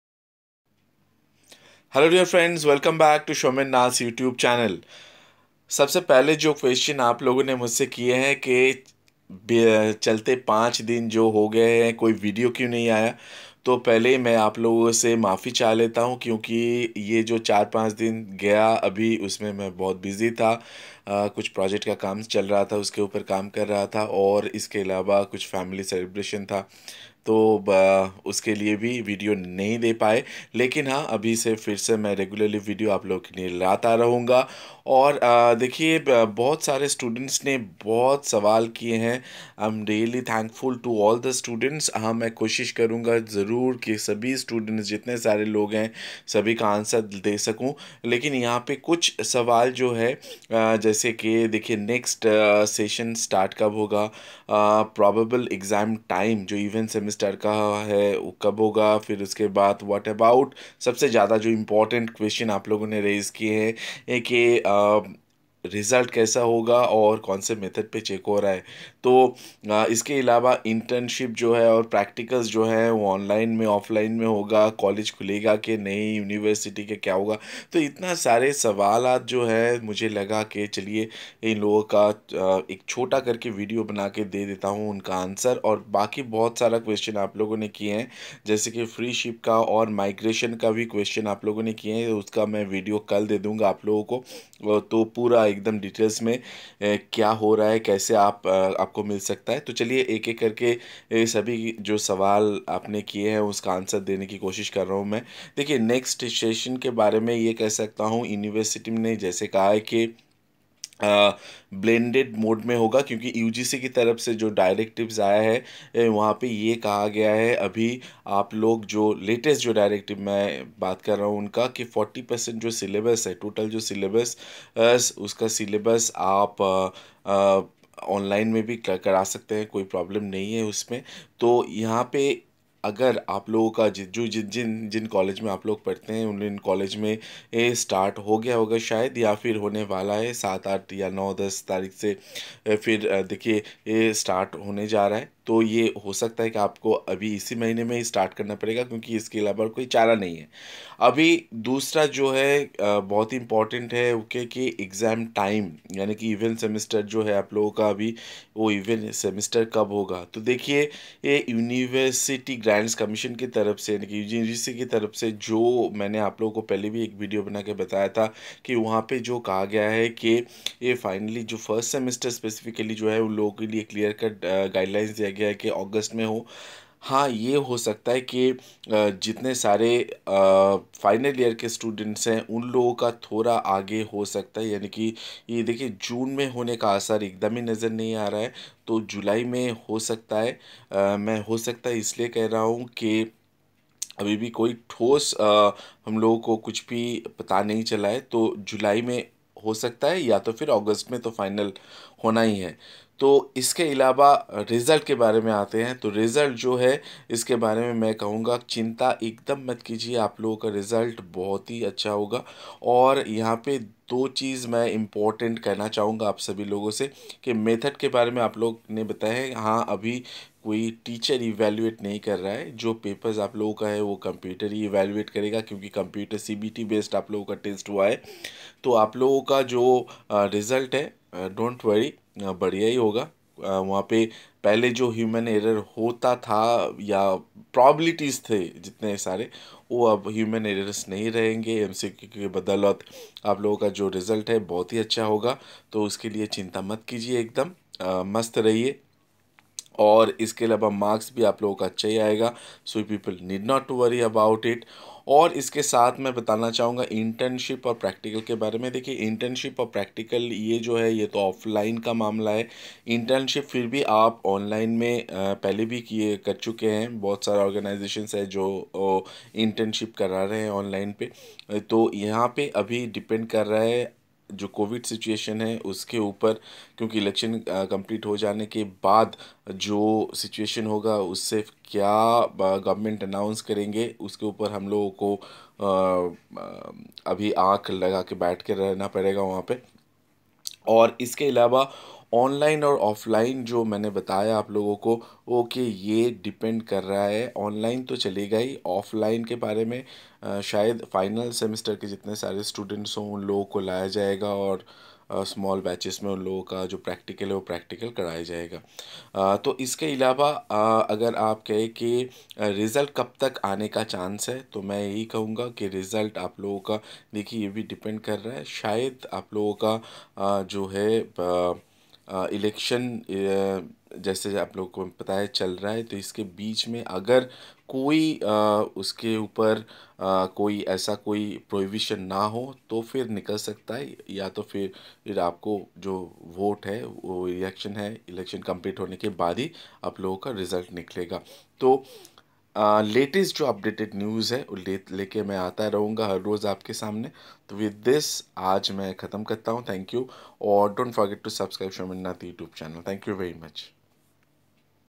MAKAUT RESULT, EVEN EXAM, ONLINE CLASSES, OFFLINE PRACTICALS UPDATES
Online Examination For All West Bengal University UG & PG Students Fully Explained:
Many a times students asked me about online examination methods. Now it is really hard to give answer in a concise manner to the comment box. Since, 3rd february' 2021, Education Minister of Government of West Bengal officially announced that all the upcoming University examinations will be held in online mode, number of questions raised by the students about the pattern of it. Here in my video, I tried to answer all the questions with my limited ability and knowledge, also about webcam on and proctoring software on mode. Opinions are purely of my own. Expecting, students are going to get benefitted from the video.

See the world of difference in new age information. Right news at right time. All the very best.

kalyani university,
jadavpur university,
burdwan university,